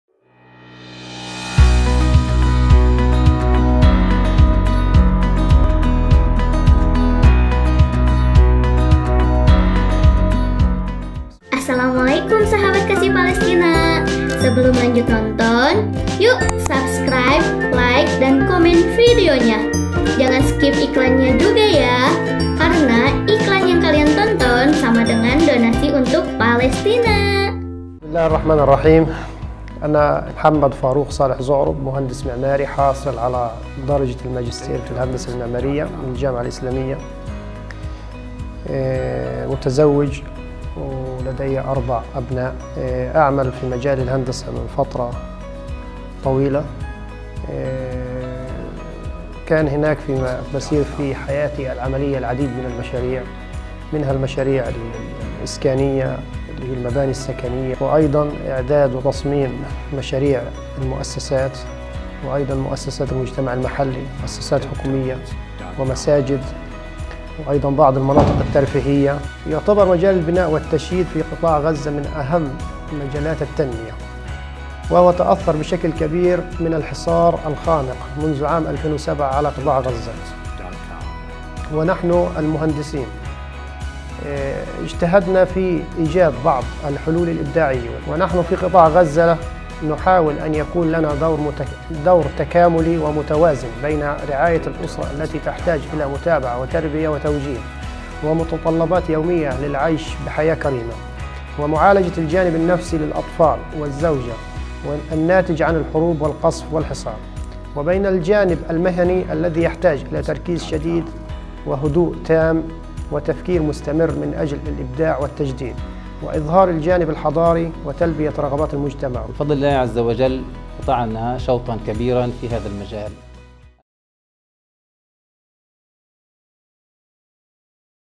Cerita dari Sang Arsitek tentang Masjid Istiqlal Gaza (Profil dari Sang Arsitek) - Kasih Palestina
Cerita dari Sang Arsitek tentang Masjid Istiqlal Gaza - Profil dari Sang Arsitek

Sahabat Kasih Palestina selama ini penasaran gak sih siapa arsitek dibalik megahnya mahakarya Indonesia (Masjid Istiqlal Indonesia)?

Meski Arsitek memiliki tugas untuk menggambar rancangan suatu bangunan, bukan berarti tugasnya sebatas ‘tukang gambar’ lho! Seorang Arsitek bisa membantumu menuangkan ide dan keinginan ke dalam gambar. Nah, gambar ini yang akan memudahkan kontraktor untuk mewujudkan konsep dan keinginan menjadi bangunan. Dalam hal ini, Arsitek akan memberikan masukan supaya bangunan bisa berfungsi secara optimal dan nyaman. Selain itu, Arsitek bertanggung jawab dalam berbagai prosedur dan pelaporan yang berhubungan dengan izin bangunan dan izin penggunaan jalan.

Arsitek harus berperan dalam proses kreatif untuk mewujudkan tata ruang dan tata massa supaya memenuhi tatanan kehidupan masyarakat juga lingkungannya.

Nah gimana nih profil dari arsitek masjid istiqlal indonesia? apakah Masjid ini projek pertama beliau? Jadi yuk cek video nya biar kenal dengan sang arsitek.